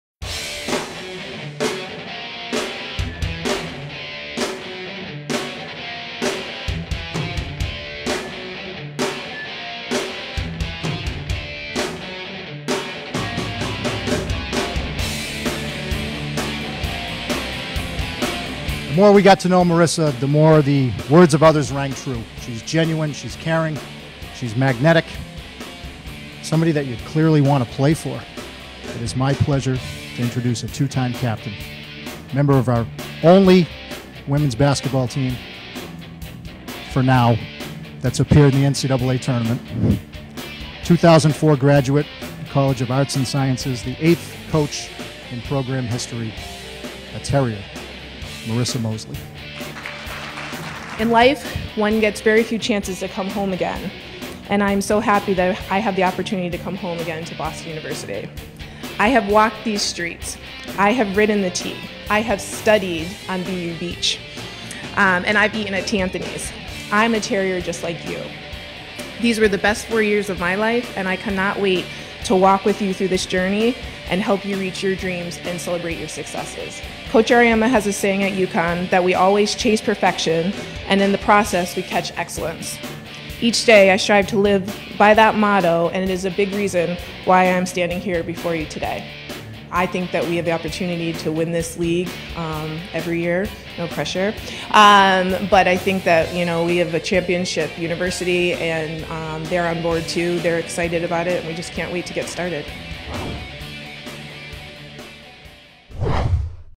Boston University Introduces Marisa Moseley as New Women's Basketball Coach | Stadium
Boston University introduced former player Marisa Moseley as the new women's basketball coach on Wednesday. Moseley spent the last nine seasons working for Geno Auriemma at UConn.